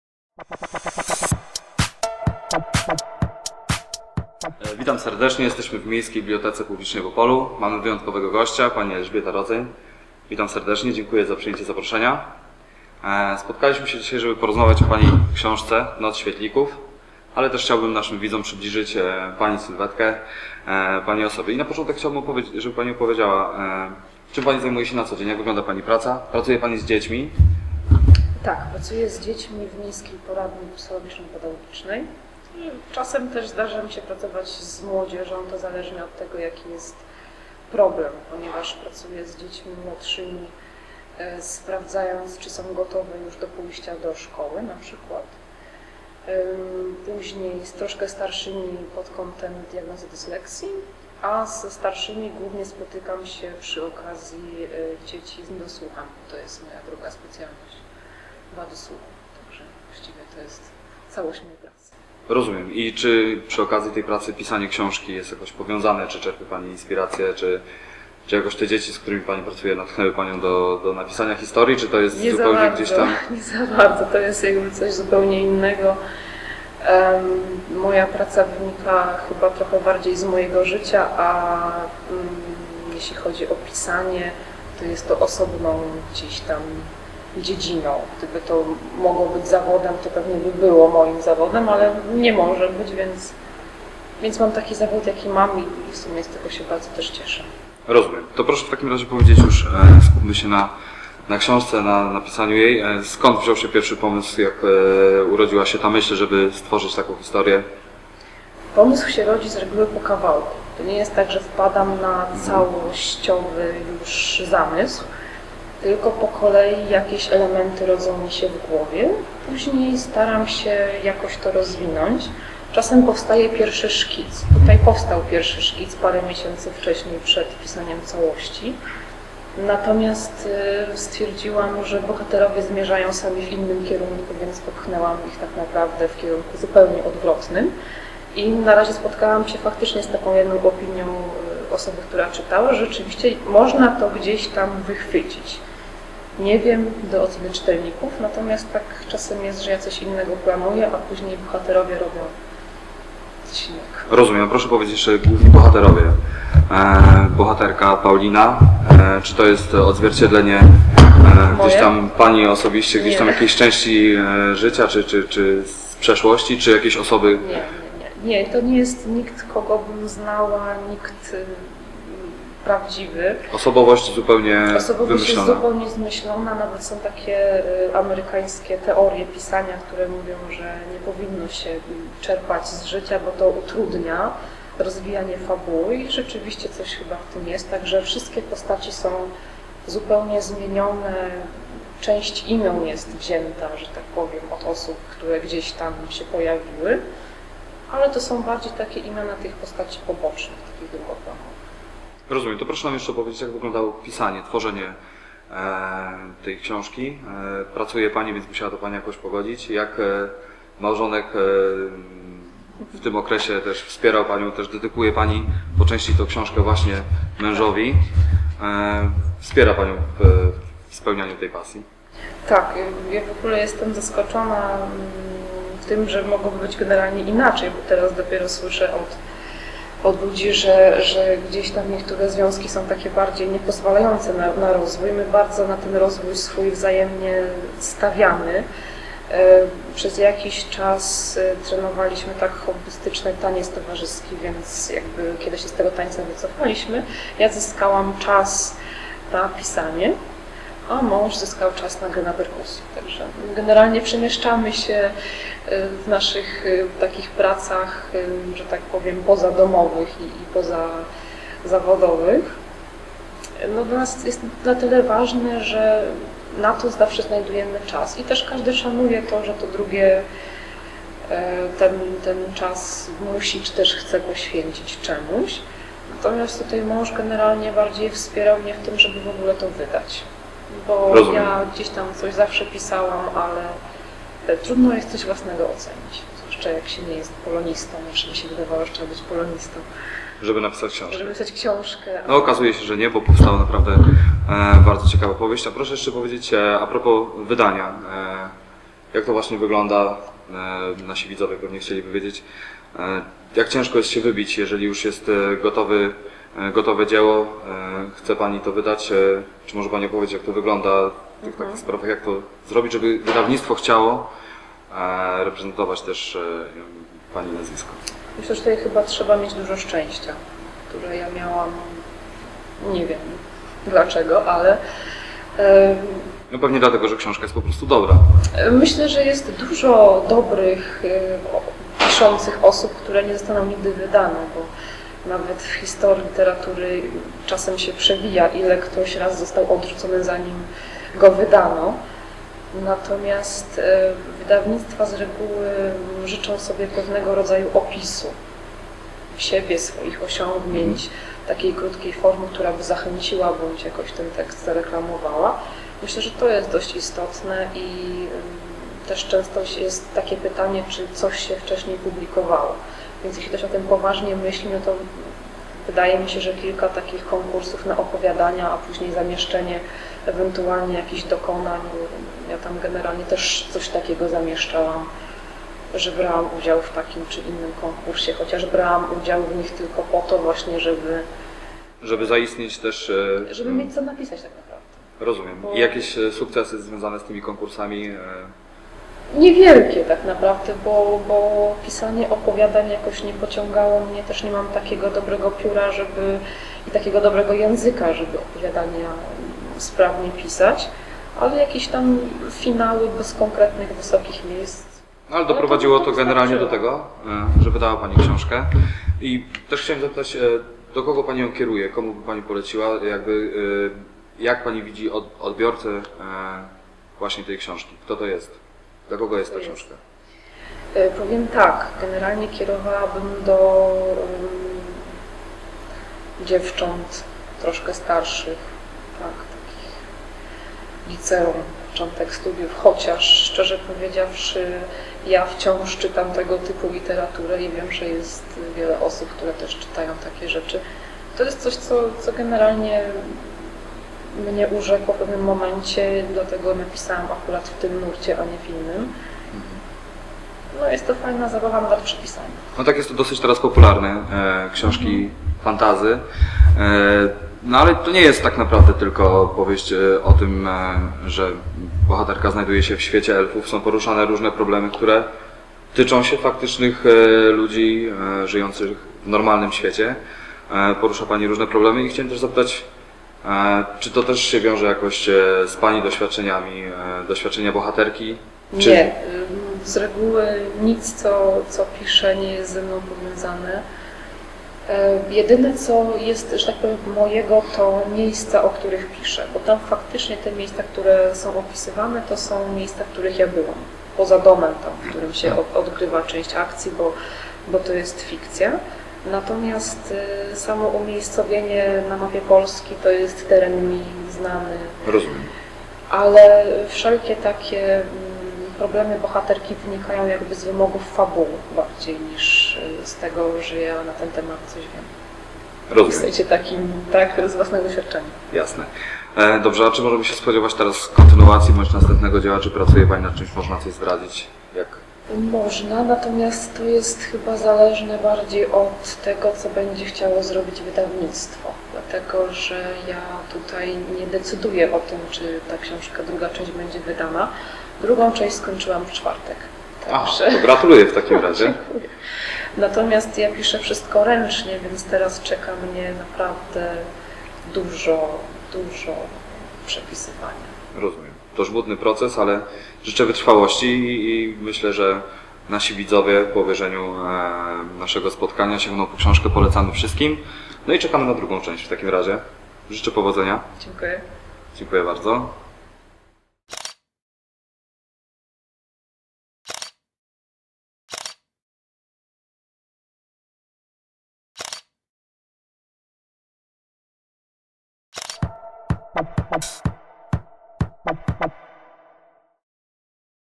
Wywiad z pisarką Elżbietą Rodzeń
Wywiad z pisarką Elżbietą Rodzeń, autorką książki " Noc Świetlików".

Kiedy magia wkracza do twojego życia, nic nie jest takie jak przedtem!